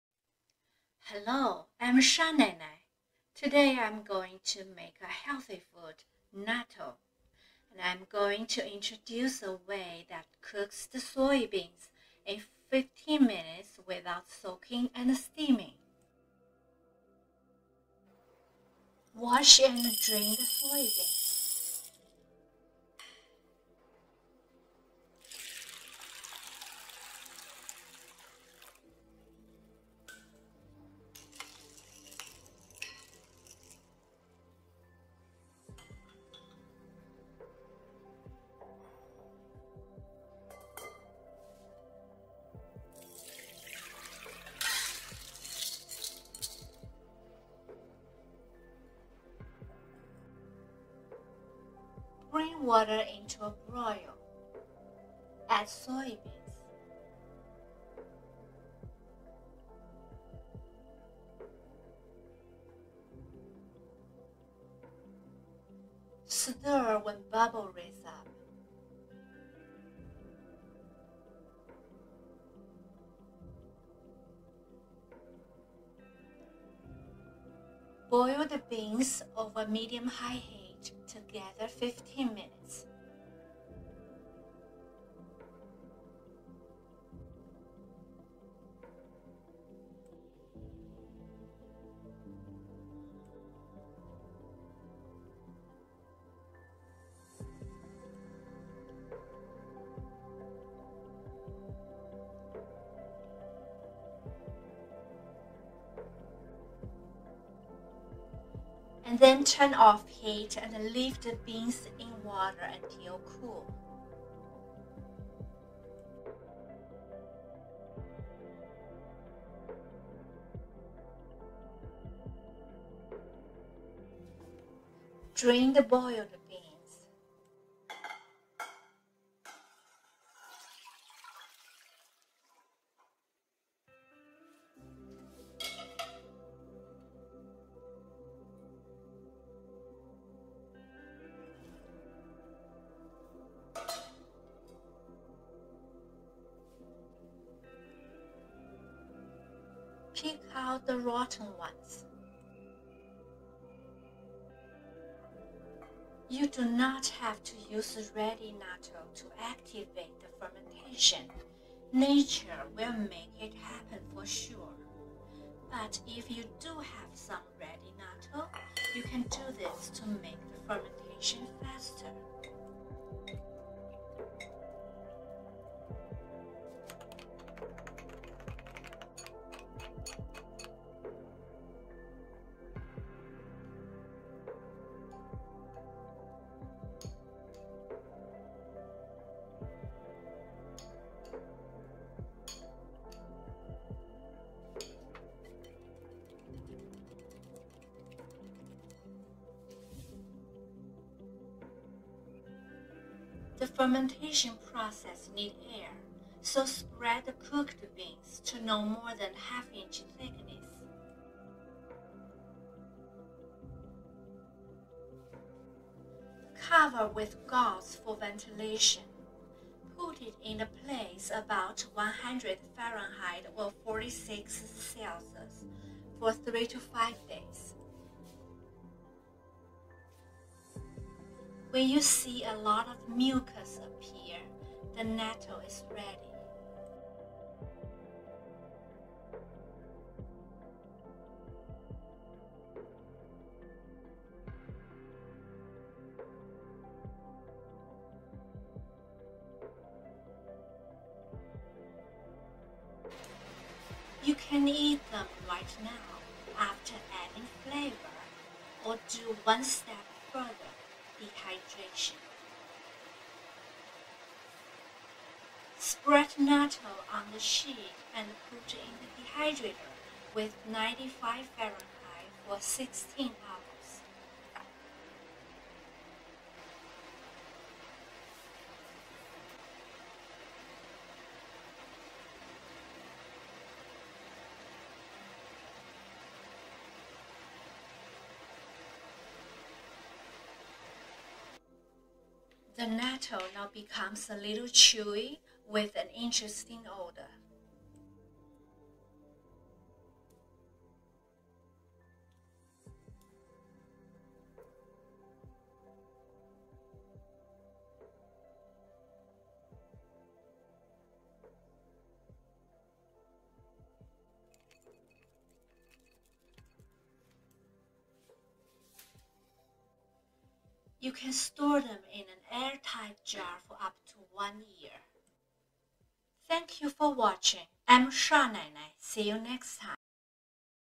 DIY Natto no yeast added - super healthy food! 自製納豆 做成陰豆豉 乾爽美味
I am going to introduce a way that cooks the soybeans in 15 minutes without soaking and steaming.

Directions

1. Wash and drain the soybeans.
2. Bring water into a broil. Add soybeans. Stir when bubbles raise up. Boil over medium high heat for 15 minutes. Turn off heat and leave the beans in the water until cool.
3. Drain the boiled beans.
4. Pick out the rotten ones.
5. You do not have to use ready-made natto to activate the fermentation. Nature will make it happen for sure. But if you do have some ready-made natto, you can do this to make the fermentation faster.
6. The fermentation process need air. So spread the cooked beans to no more than ½ inch thickness.
7. Cover with gauze for ventilation. Put it in a place about 100f or 46c for 3-5 days.
8. When you see a lot mucus appear, the natto is ready.
9. You can eat them right now, after adding flavor; or do one step further – dehydration. Spread natto on the sheet and put in the dehydrator with 95°f for 16 hours.
10. The natto now becomes a little chewy with an interesting odor. You can store them in an air tight jar for up to 1 year.